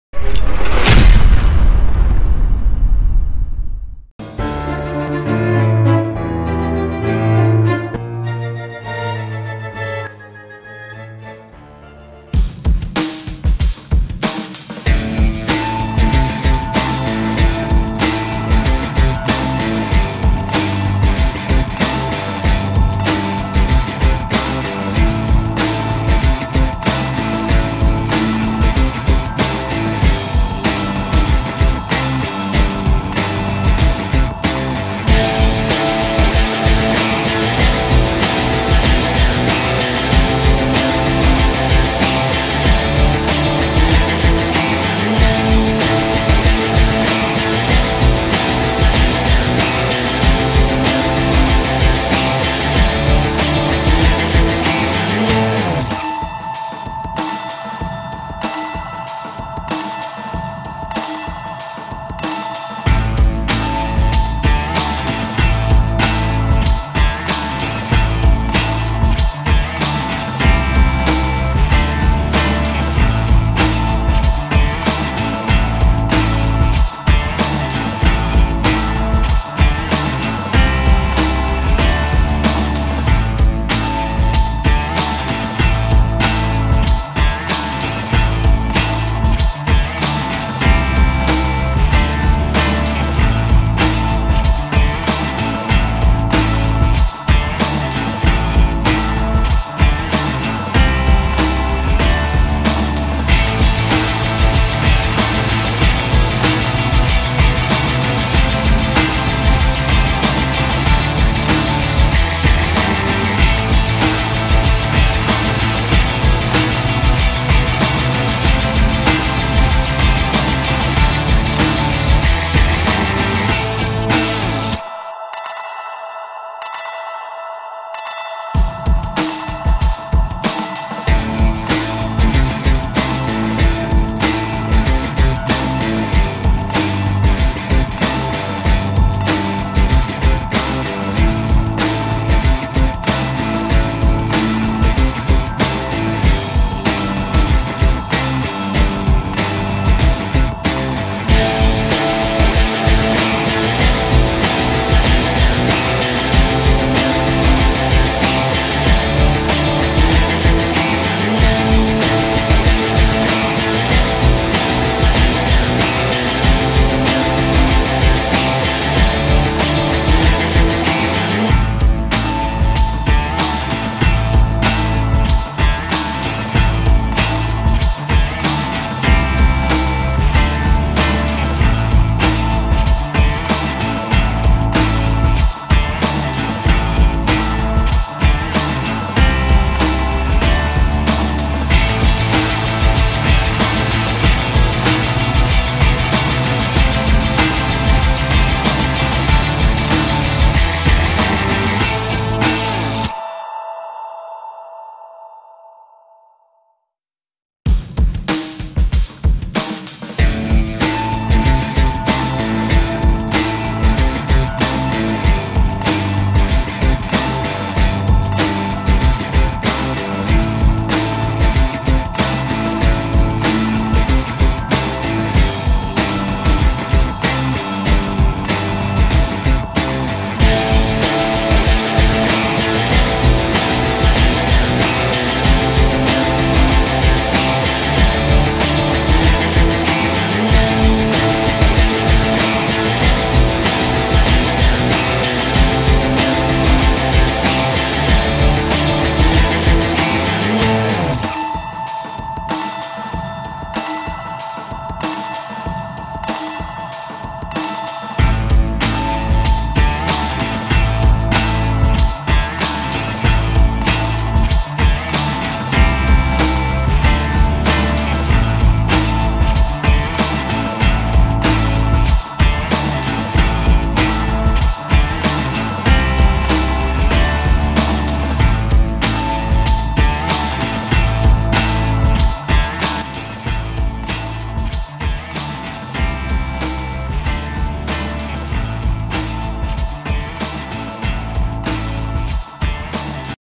5 Conditions That Soru Soru Could Take THE SOUL | One Piece 855 Review
You will see about theory Soru-Soru power based on the legends Morgan Le Fay. In the previous review, I am explain about Morgan le Fay and his unique power. Many literature said that and it has made some film. But the data was pointed to Morgan special power, power to transfer soul from the human or animals body. Dead creature is given life by the process of soul transfer was called Homonculus, if Soru-Soru power named Hommies.